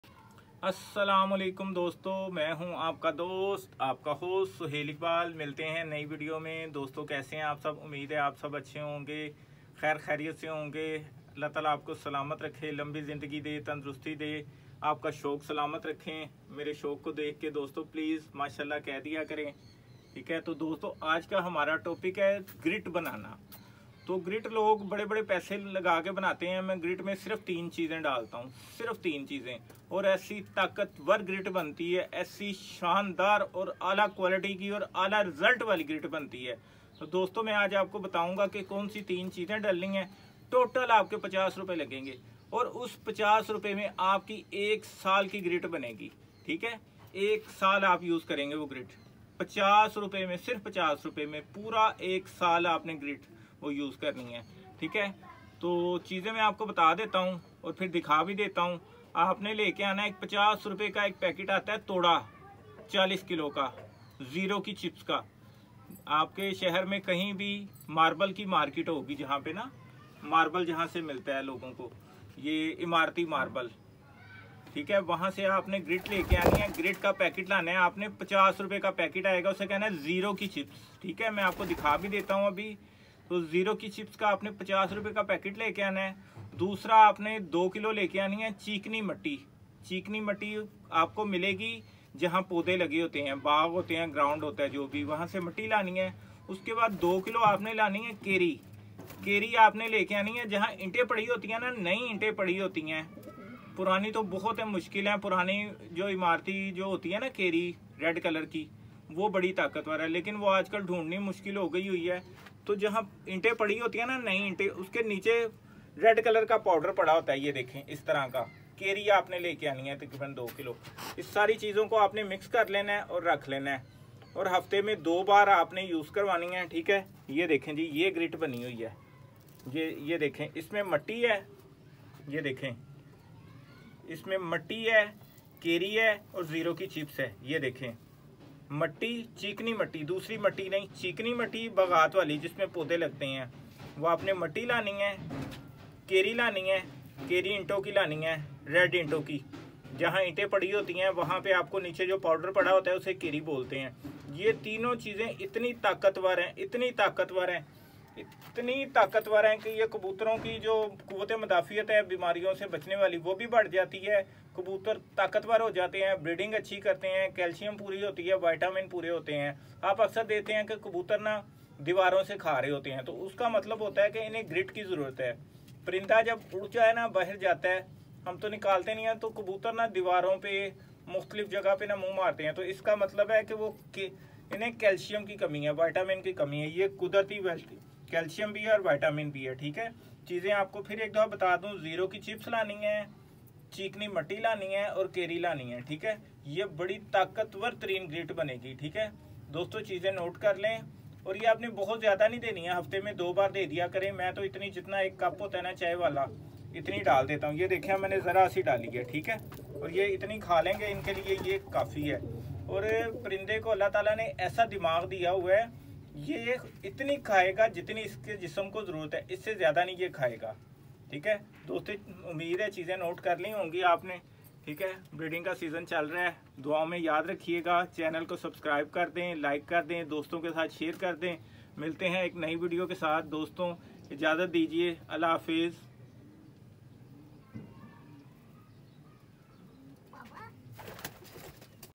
How to make Pigeon grit || kabutar ke liye mitti kase bnaye
MAKE GRIT FOR Pigeons (MITTI). कबूतर के लिए पौष्टिक मिट्टी बनाए !!
Kabootar Ki Taqat K Leay Gala Bnane Ka Tarika کبوترکو غلا لگنے کا مکمل طریقہ